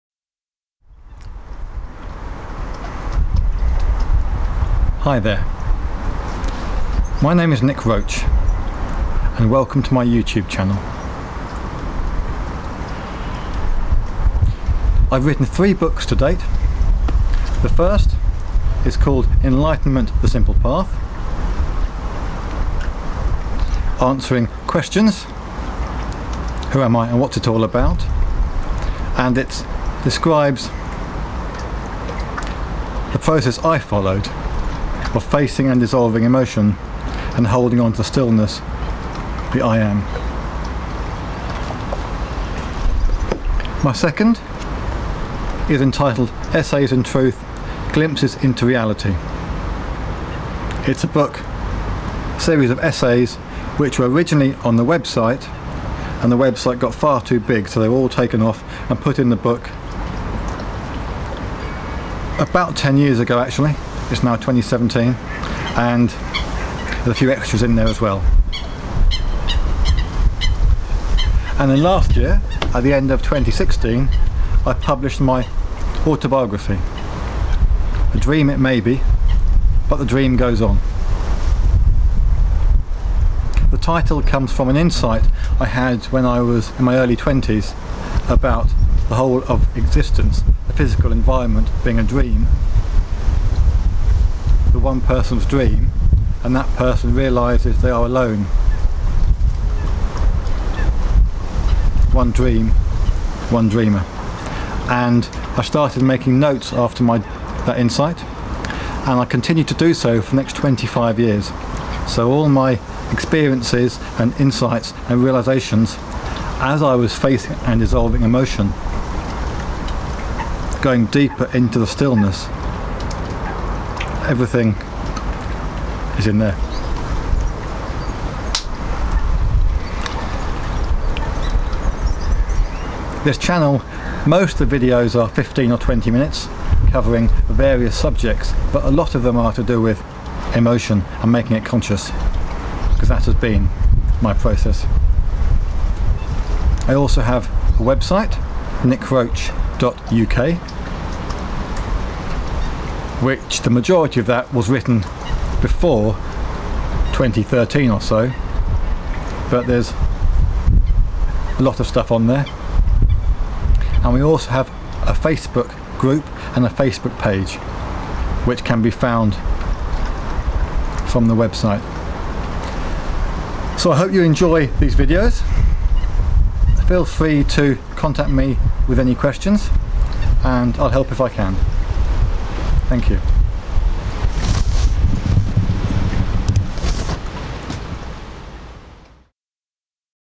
Nick Roach - author and spiritual teacher
Nick Roach, author of four books, perhaps most notably his spiritual autobiography entitled 'A Dream It May Be, but the Dream Goes on!' speaks in a series of videos on various subjects.